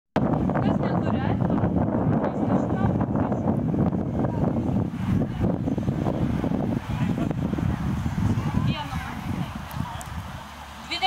Aitvarai Žirmūnuose. 4
2015 rugsėjo 19
Žirmūnai, Vilnius
Žirmūnų bendruomenės aitvarų šventė.